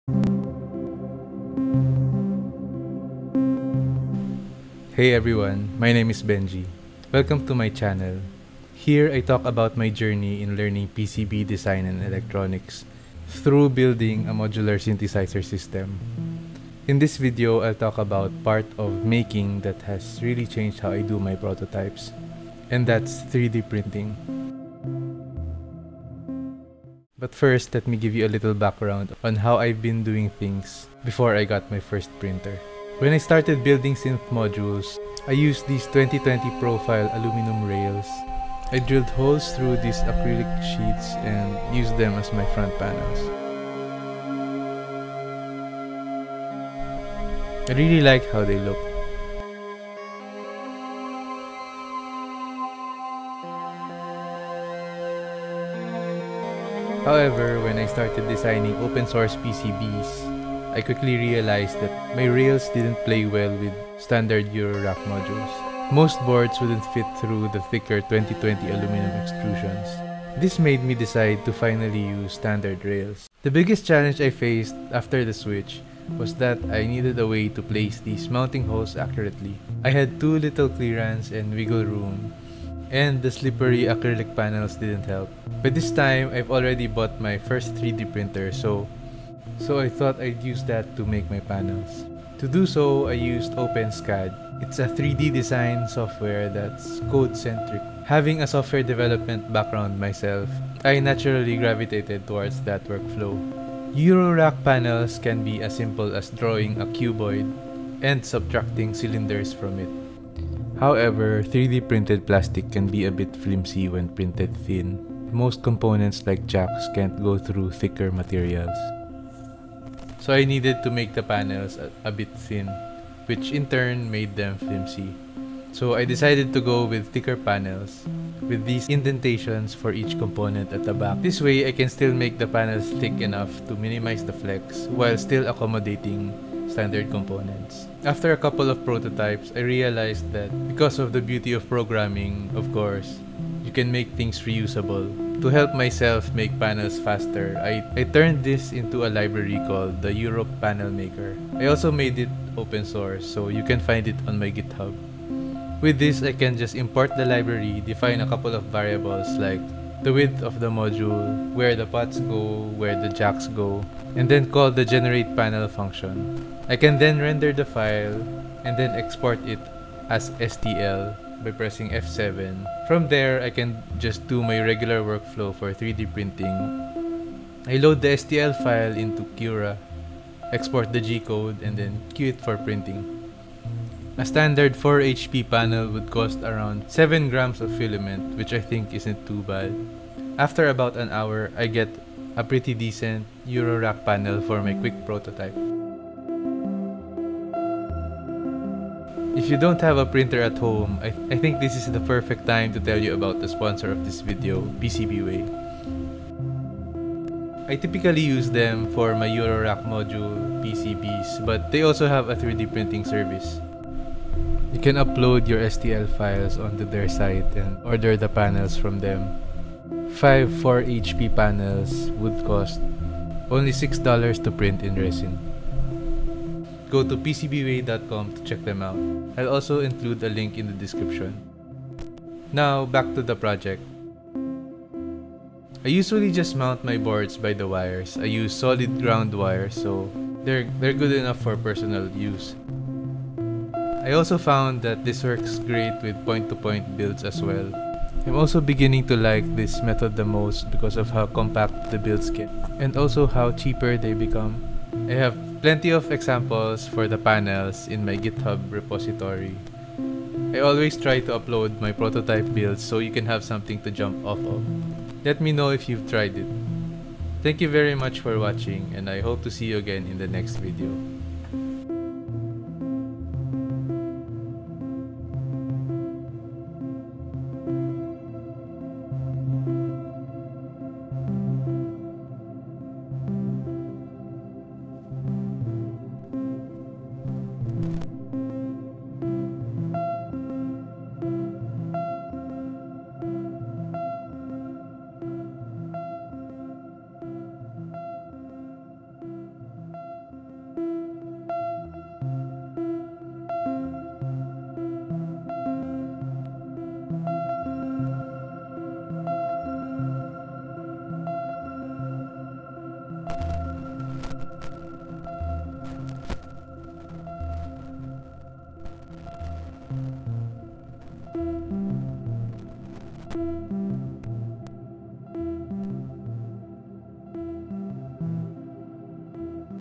I made a EuroPanelMaker library for OpenSCAD!
In case you don't have a 3D printer at home, @PCBWay offers a 3D printing service with a variety of different materials like resin, nylon, and a whole lot more! Sign-up here to get $5 free credit Thanks, PCBWay, for sponsoring this video.

Timestamps
00:00 Intro
00:26 How I've been building my prototypes
00:56 Why I need 3D-printed panels
01:30 OpenSCAD
02:17 The EuroPanelMaker library
03:17 Sponsor: PCBWay
03:54 Mounting the circuits
04:14 Examples for EuroPanelMaker
04:25 Outro